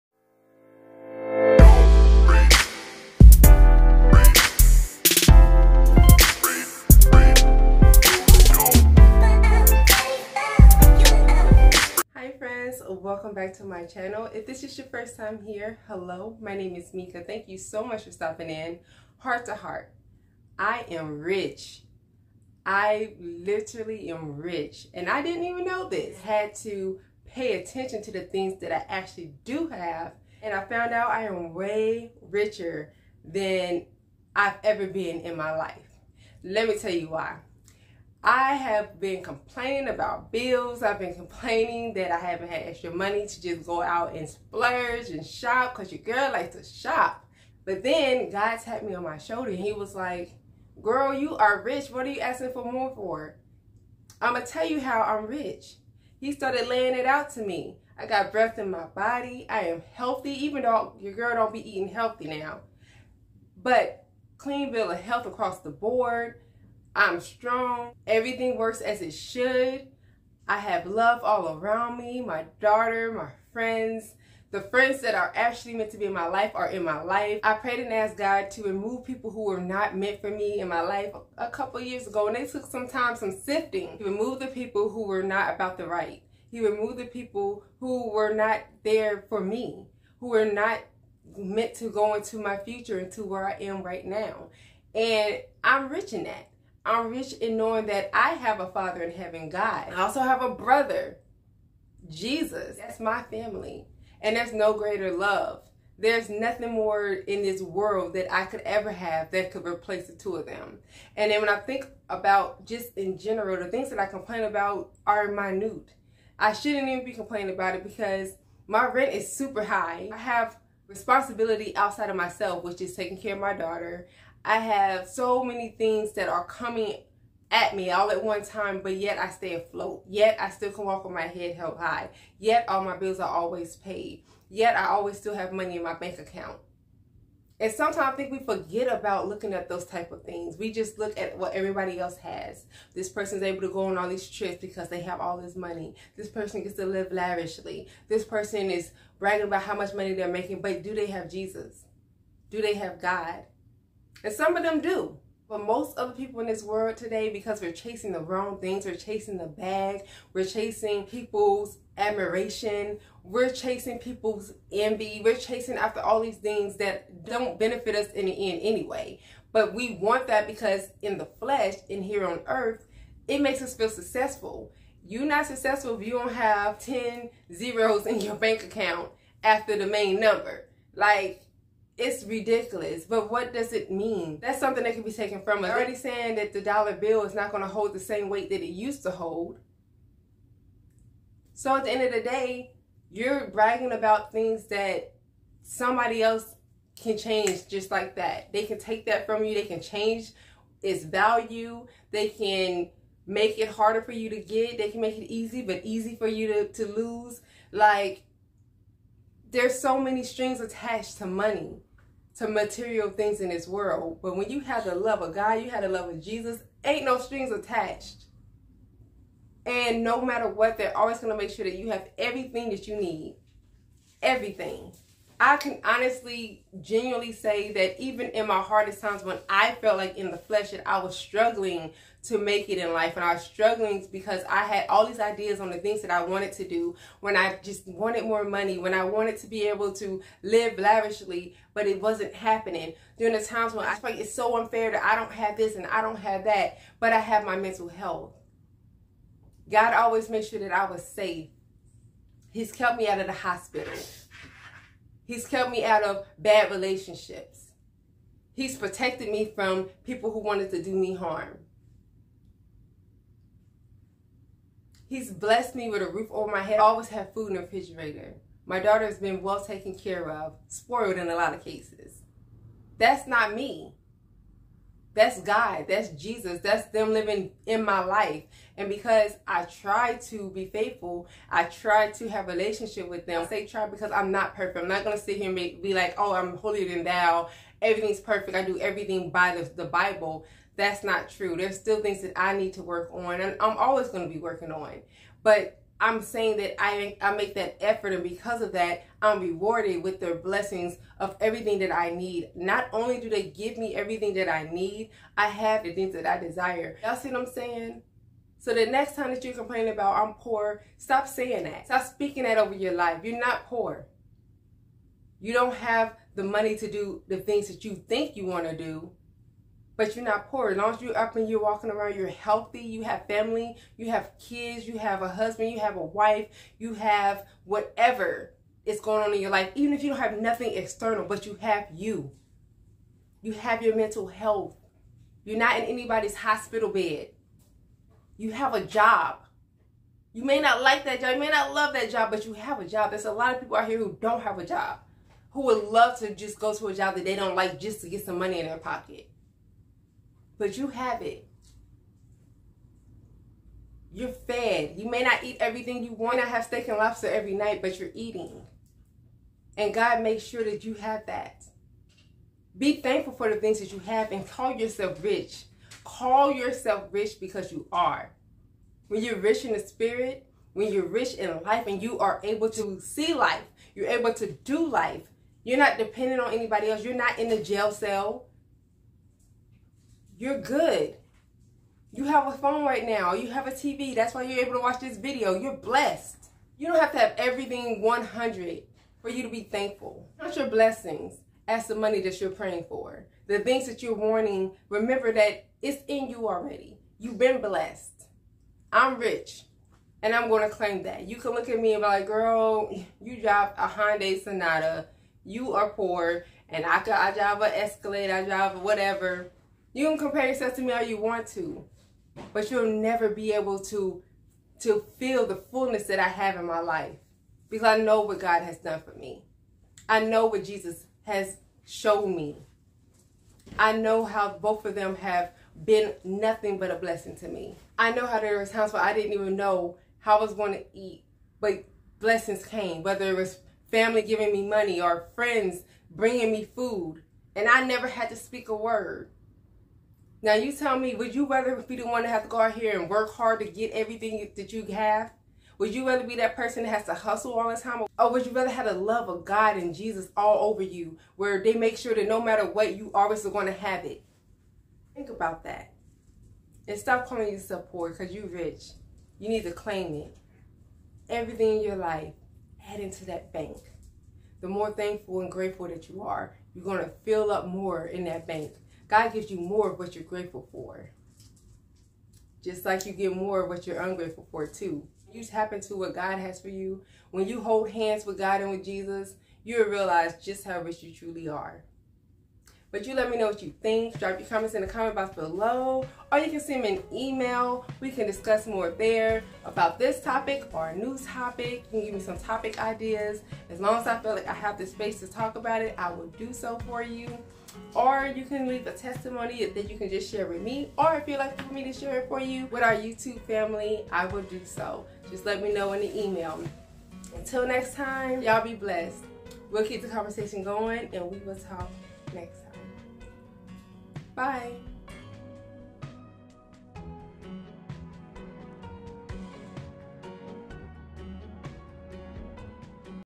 You're Richer Than You Think
In this video I give MY OPINION on what being truly rich looks like, and it doesn't look like having a big bank account.

Video upload schedule: Every Monday & Thursday @ 5 PM CST